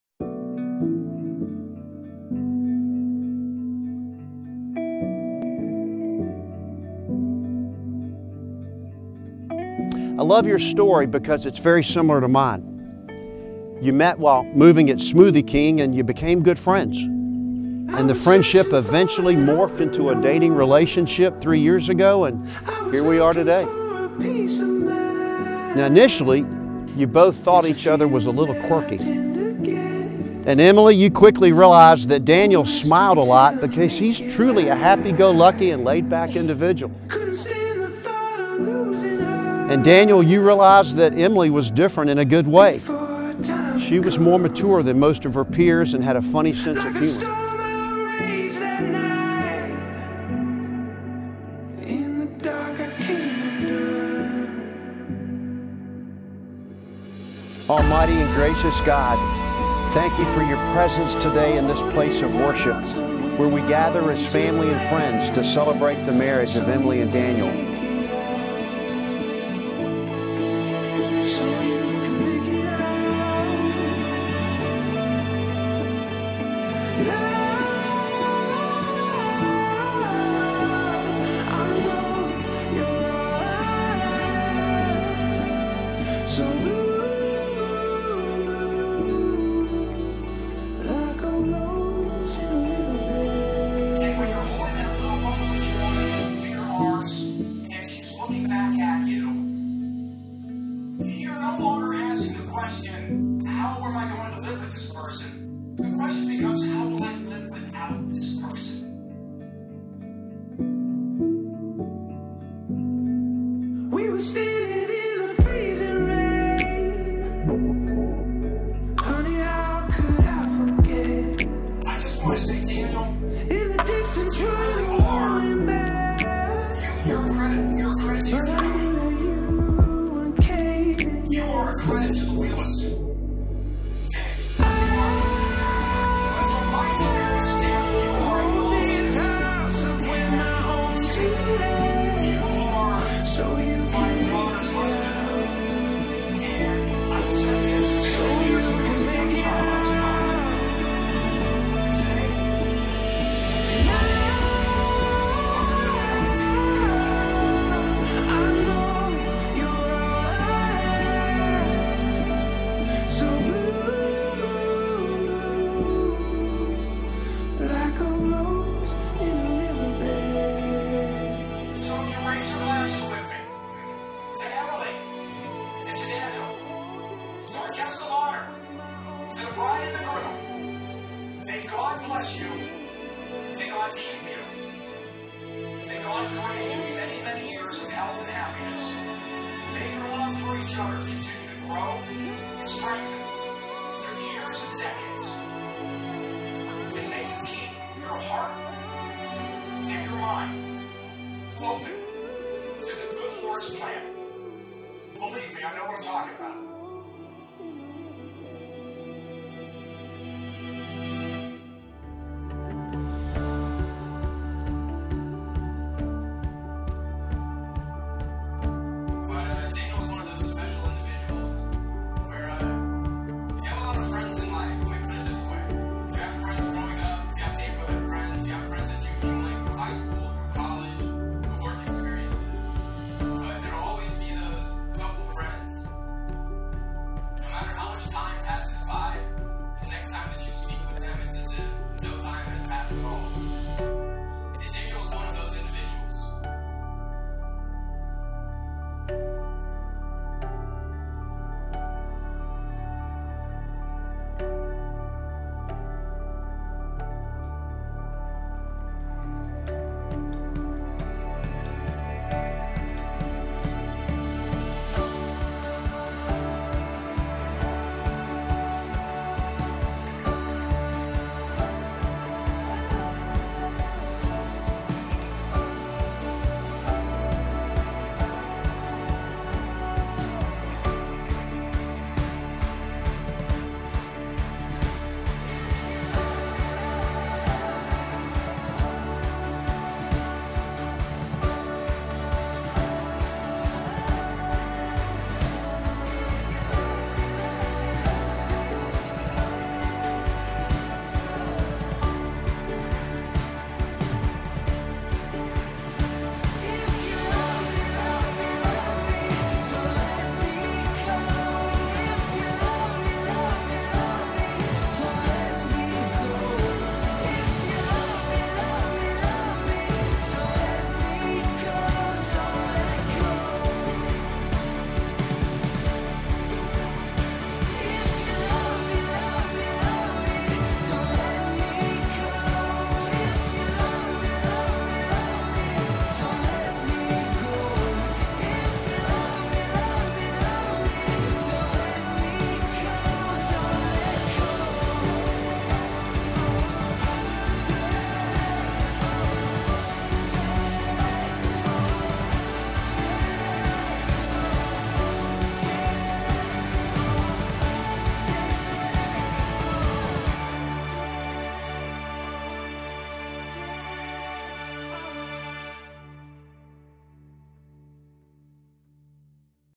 A Wedding on the Banks of the Neuse River I Daniel & Emily
It's here! Daniel & Emily are two of the sweetest individuals you will meet. Their day was shared among their friends and family on the banks of the Neuse River in Havelock, NC. It was truly a wedding with a view and an evening of joy and laughter. The afternoon rain showers fell as Daniel & Emily prepared for their turn to walk down the aisle. As the showers cleared friends and family began to arrive, and as the evening went on, a beautiful sunset filled the sky. The night was filled with dancing and laughter, as Daniel and Emily began their new life together.

Vendor Team:

Venue: Neuse Breeze Wedding Film
DJ: Generations Mobile DJ Service
Catering: The Flame Catering and Banquet Center